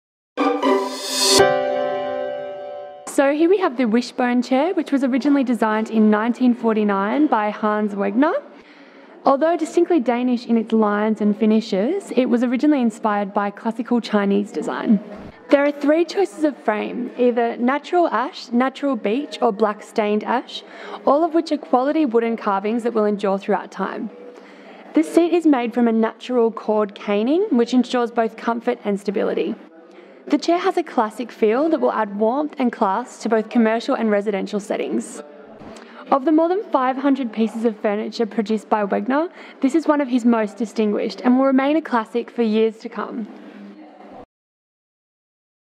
The Wishbone Chair, byBESPOEK
byBESPOEK premium reproduction of Hans Wegner’s iconic Wishbone chair is a handmade designer dining chair inspired by the timeless 1949 design. You can now buy this highest-quality designer dining chair in wood of premium quality to ensure an entirely classic style for your home at a favourable price.